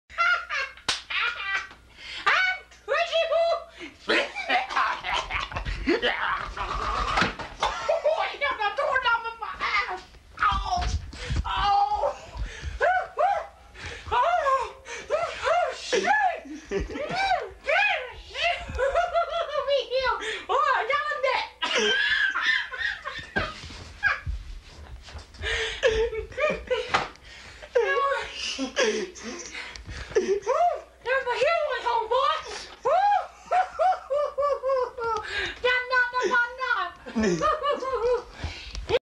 Jeff and the Doorknob
Jeff really hurts himself in this one. This is also one of the few times you will ever hear him swear. Filmed in 1992.